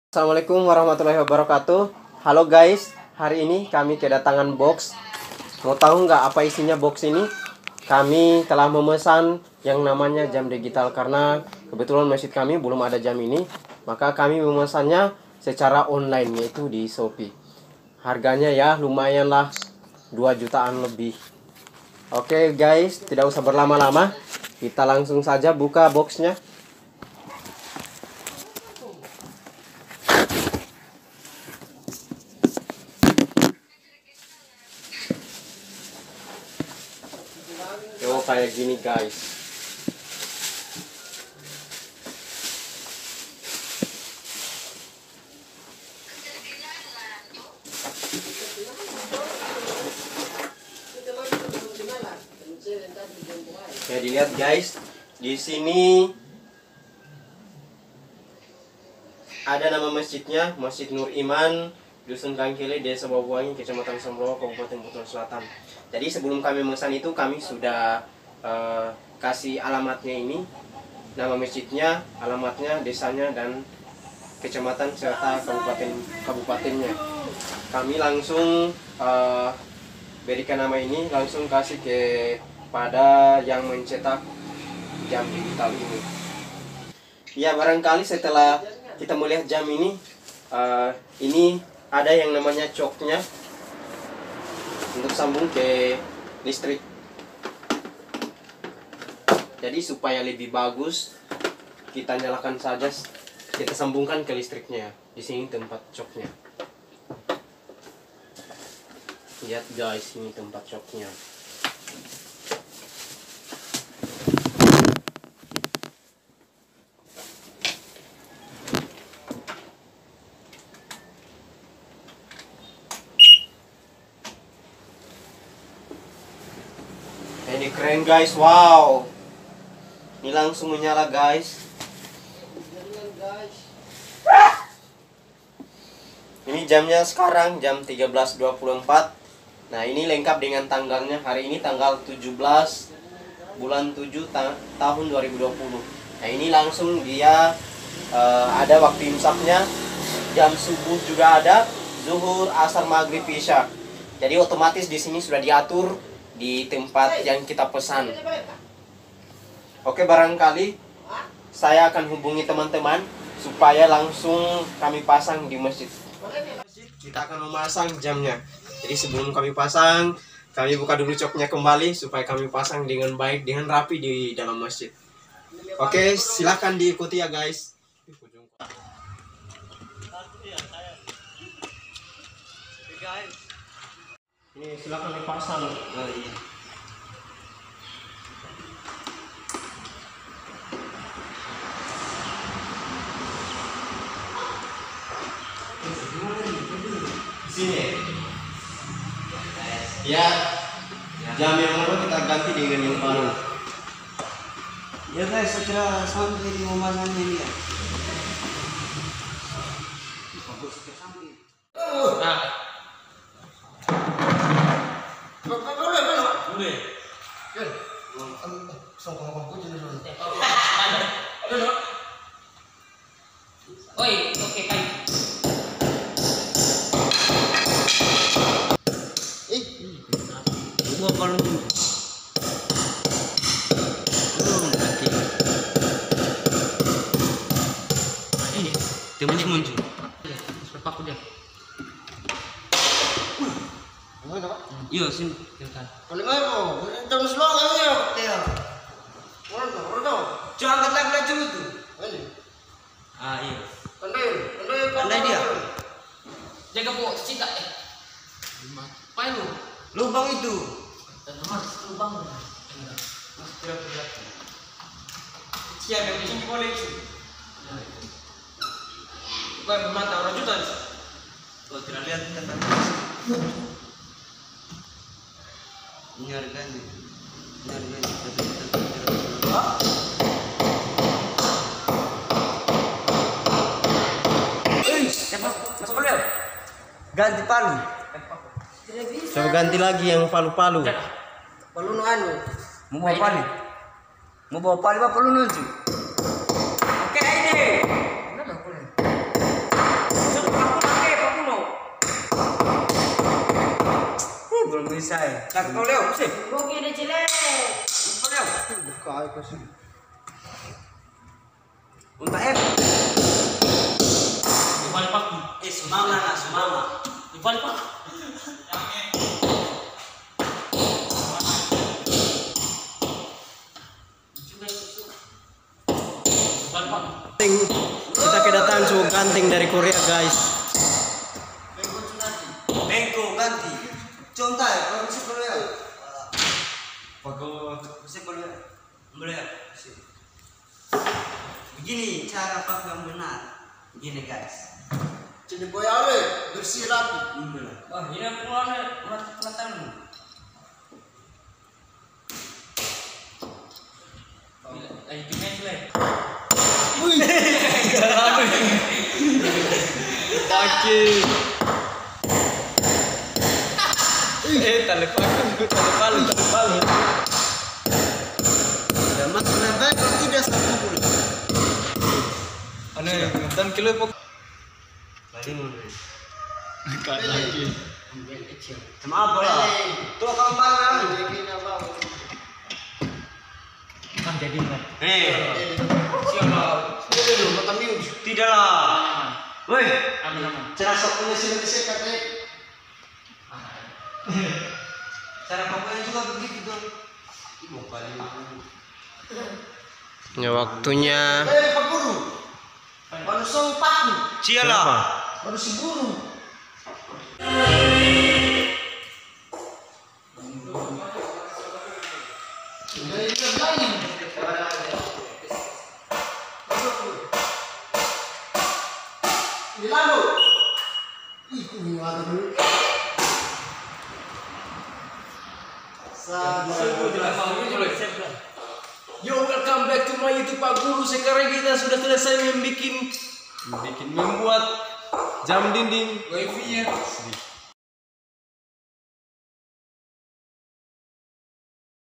Jam digital Masjid Nur Iman Wawoangi
Jam digital yang memiliki banyak keunggulan yakni setiap  waktu sholat alarmnya akan bunyi, mau iqomah bunyi alaram, dll. lengkap dengan jadwal sholat 5 waktu.

jam digital dinding,
jam dinding digital led,
jam dinding led digital,
jam dinding digital terbaik,
jam dinding digital murah